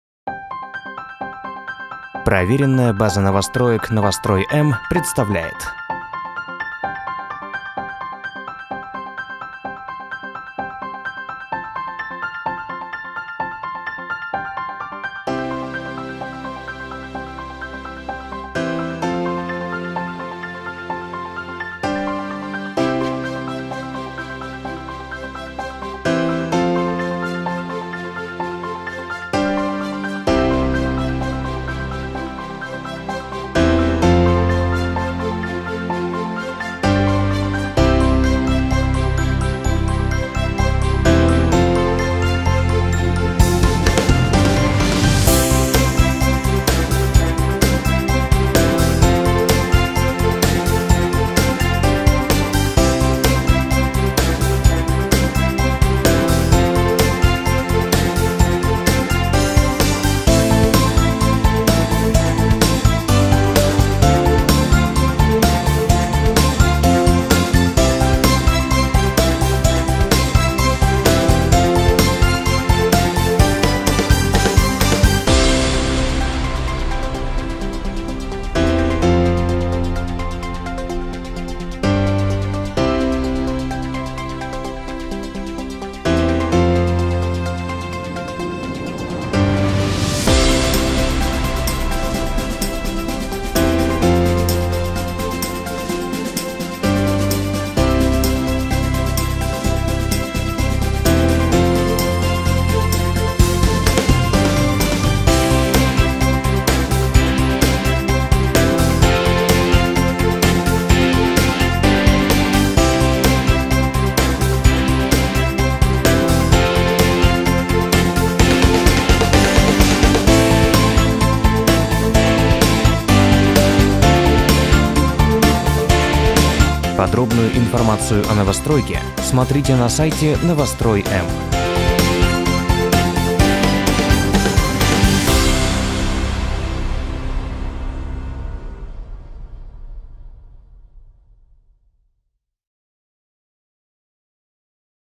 ЖК «Маяковский» от девелопера Tekta Group (аэросъемка: июль 2017 г.)
Подробные схемы проезда к жилому комплексу, фото со стройки, а также строительную документацию смотрите на сайте Novostroy-M.ru

ЖК «Маяковский» возводится в САО г. Москвы, на границе Войковского и Головинского районов, в 11 минутах ходьбы от станции метро «Водный стадион».

Девелопер проекта — компания Tekta Group. Информацию о девелопере можно прочитать на сайте Novostroy-M.ru

Срок сдачи: IV кв. 2018 г.
Тип проекта: монолит
Этажность: 36
Комнатность: 1, 2, 3, 5
Площадь квартир: 40 — 136 кв. м